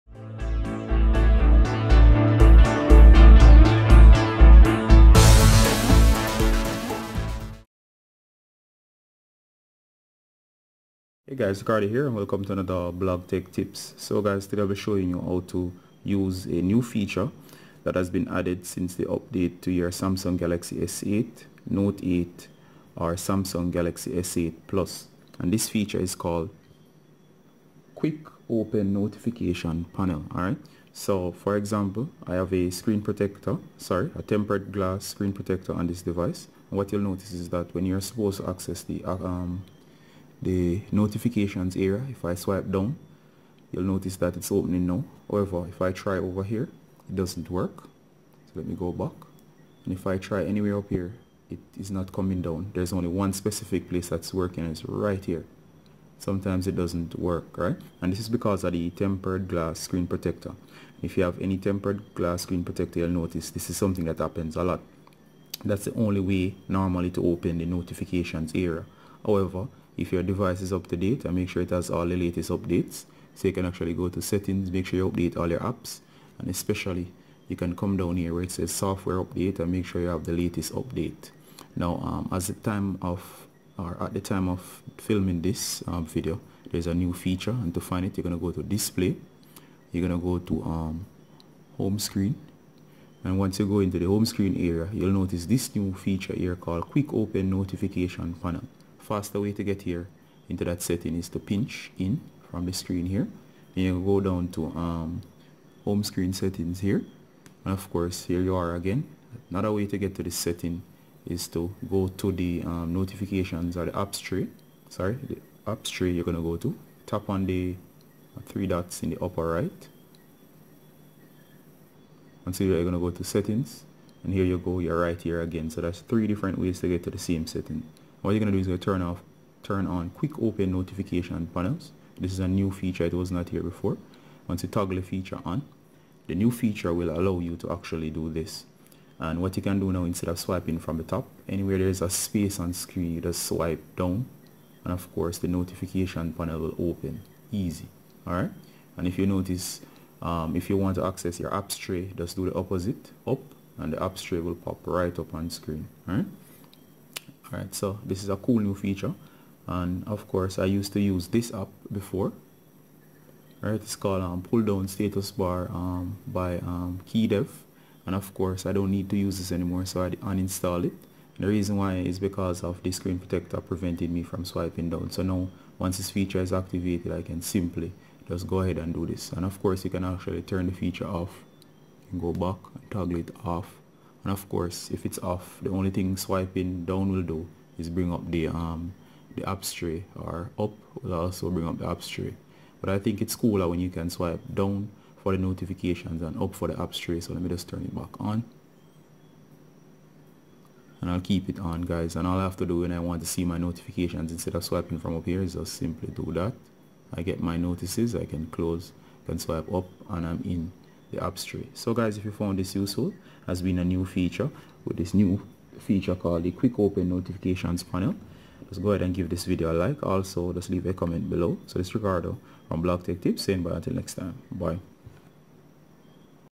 QUICK OPEN Notification PANEL New Feature/Gesture Galaxy S8,S8 Plus & Note 8
With the latest updates Samsung has added a new Quick-open Notifications panel gesture or feature that you must activate to easily access your notifications area by swiping down from the home screen. This works great if your Screen protector or tempered Glass screen protector is affecting your ability to swipe from the top to access notifications.